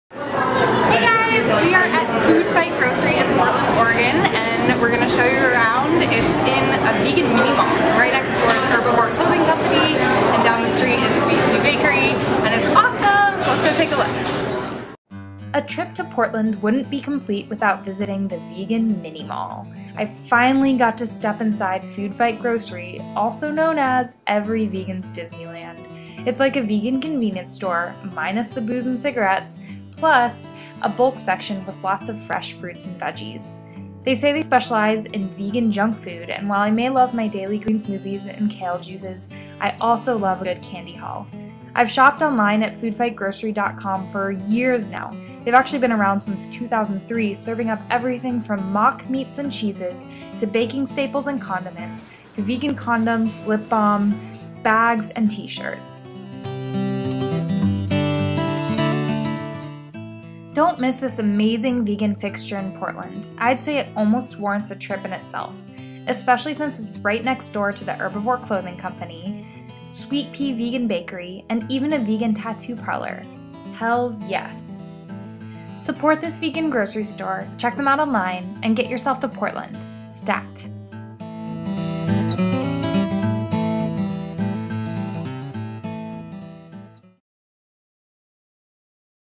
Food Fight! Grocery in Portland ♥ Vegan Paradise!
That's right. There is a vegan mini-mall in Portland, Oregon. This video shows you a peek inside the totally vegan grocery store Food Fight Grocery!

Can't make it to Portland? Check them out online (just be ready to fall in love)